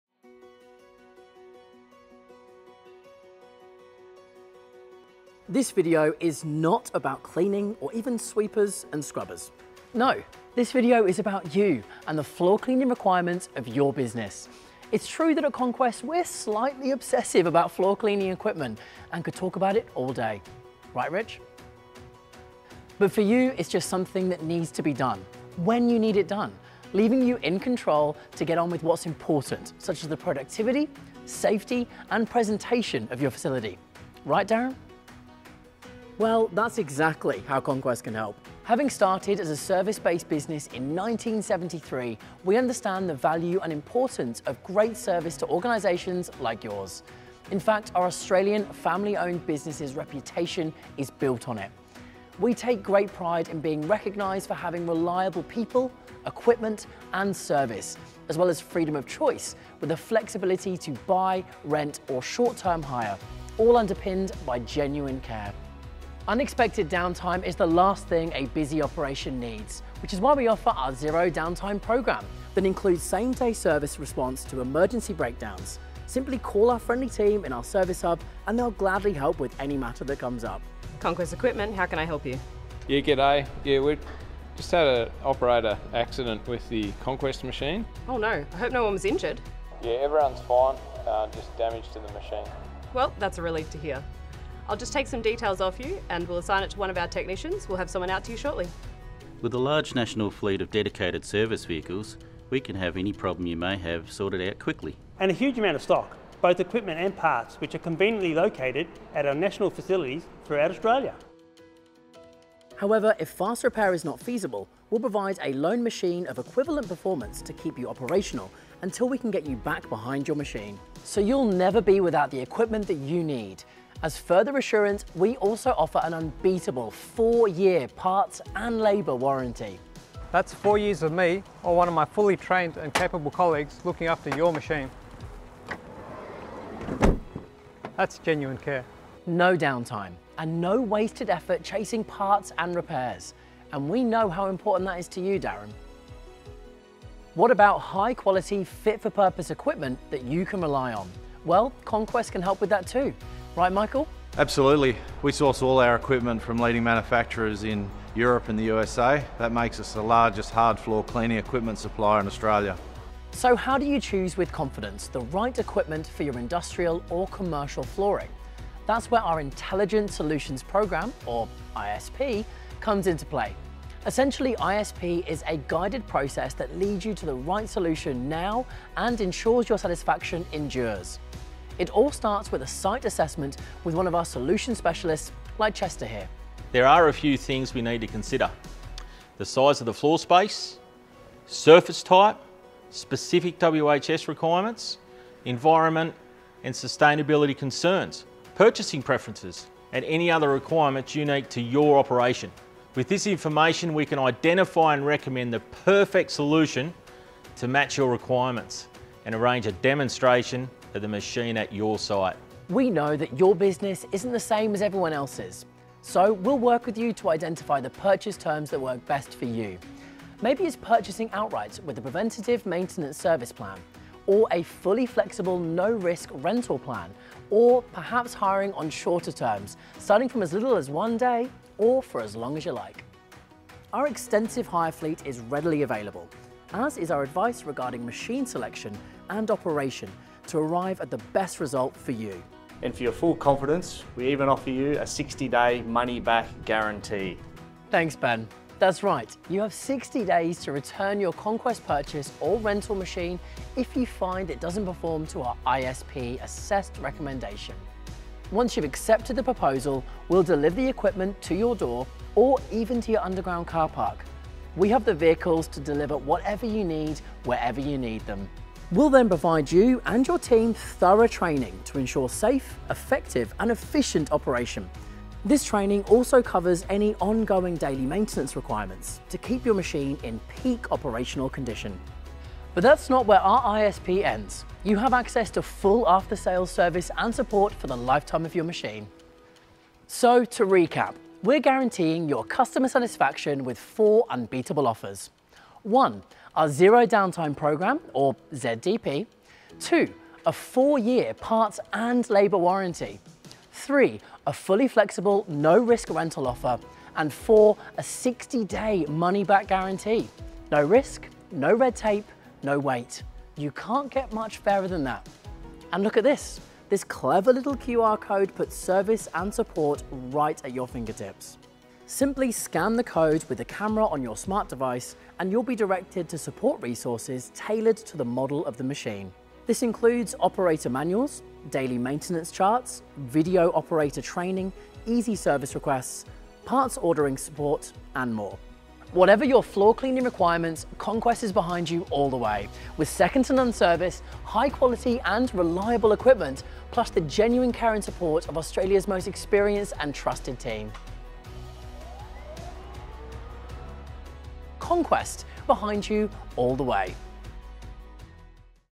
Conquest - Behind You All The Way
As an Australian family-owned business, at Conquest, we recognise the significance of quality service in floor cleaning. Our commitment goes beyond sweepers – with reliable professionals and equipment, ensuring the unique floor cleaning requirements of your facility are met with precision.

Experience freedom of choice in purchase options, tailored to suit your needs and budget. At Conquest, genuine care for our customers is at the core of what we do. Our team provides you with the right solution backed by our unparallel after sales service and support.

Learn more about our Intelligent Solutions Program (ISP) as we showcase how Conquest applies cutting-edge technology and a wealth of industry knowledge to guarantee the right floor cleaning solution for your business.

Elevate your cleaning standards with Conquest, where reliability, freedom of choice, and genuine care guarantee we’re behind you all the way.

To learn more contact our friendly team on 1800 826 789 or visit conquestequipment.com.au